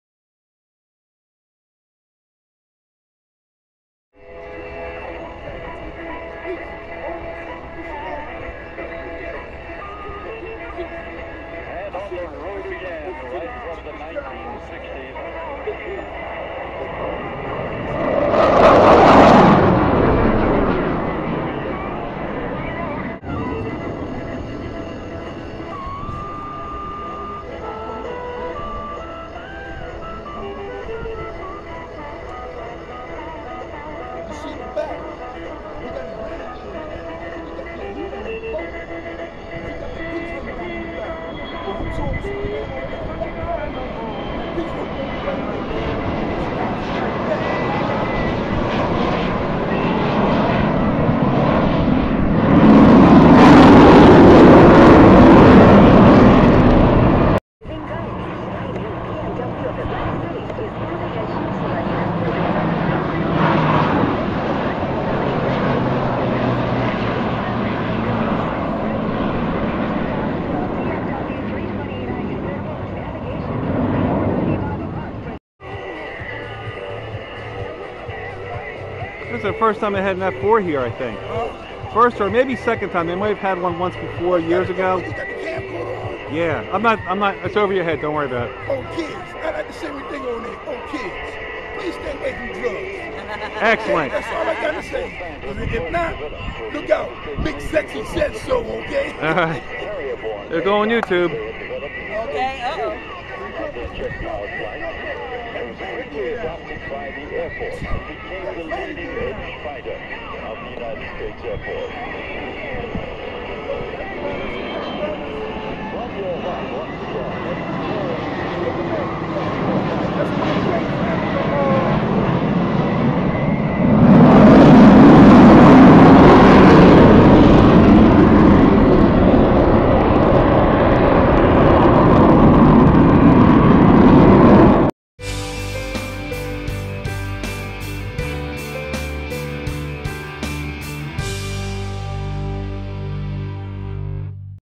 F 4 Phantoms in AC
The F-4 Phantom Demo at The Atlantic City Airshow Thunder over the Boardwalk August 17th, 2012